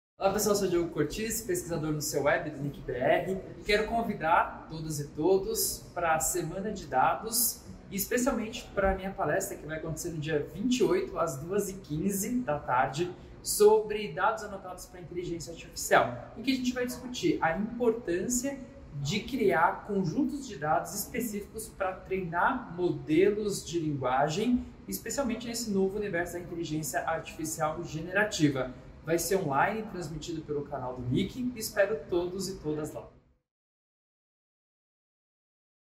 Acompanhe os painéis do NIC.br na Semana de Dados BR!
Neste vídeo, o analista de projetos do Cewebbr | NICbr, Diogo Cortiz, convida o público para acompanhar a Semana Dados BR! Ele apresentará o painel Dados Anotados para Inteligência Artificial, que será transmitido ao vivo pelo canal do NIC.br, no dia 28/9, às 14h15. Não perca!

A Semana de Dados é uma realização do NIC.br em parceria com a Controladoria-Geral da União (CGU), o Ministério da Gestão e da Inovação em Serviços Públicos (MGI) e apoio do Serpro e da Dataprev.